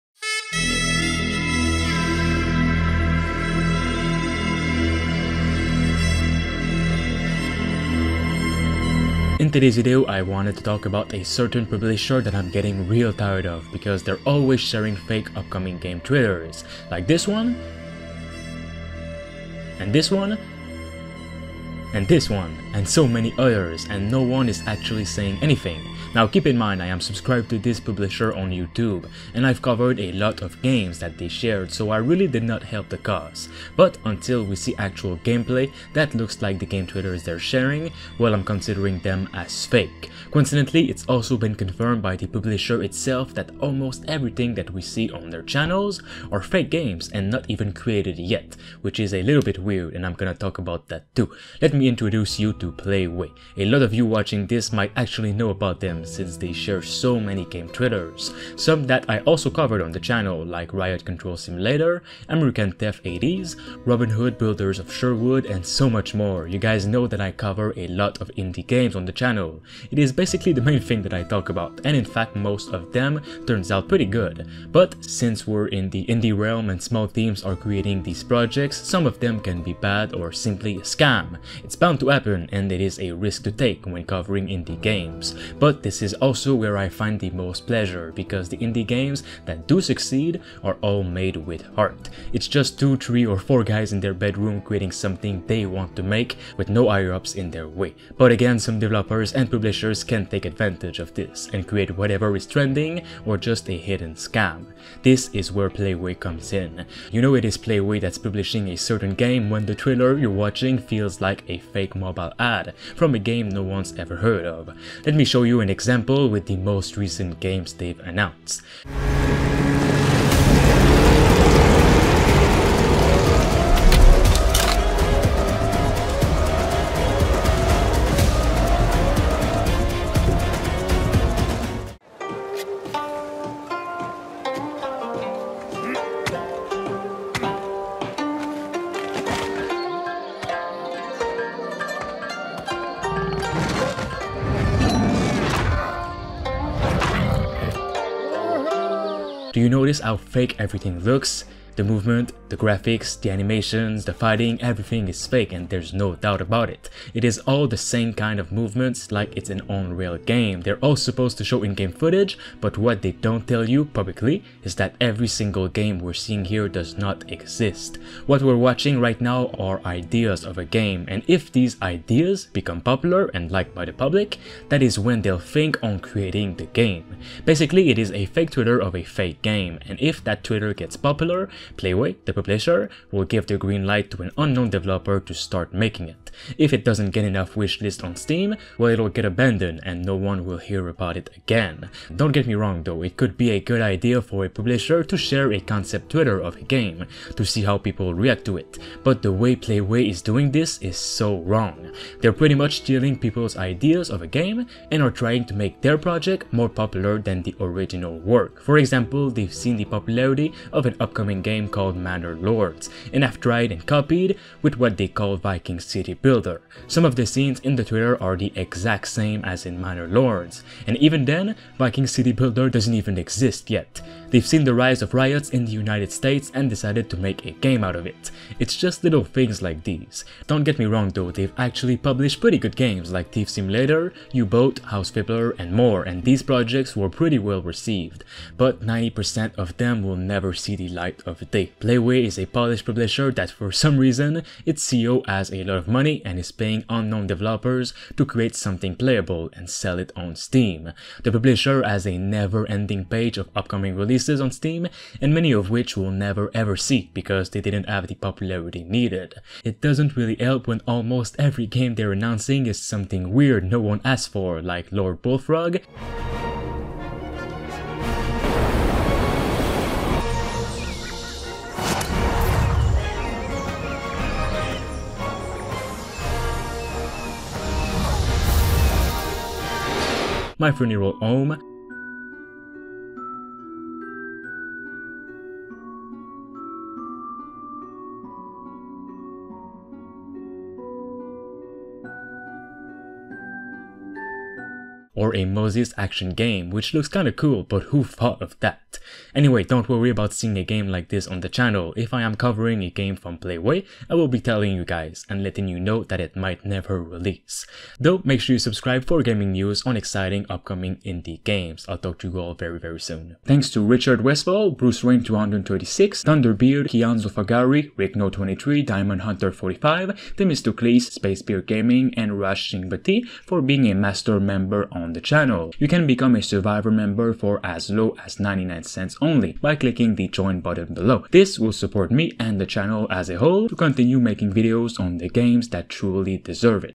This game publisher is sharing fake games... How is this allowed?
We really need to talk about Fake games and scams in the gaming industry, especially with one publisher. There's so many upcoming games, with a full trailer and description, that are not even real and it's not cool!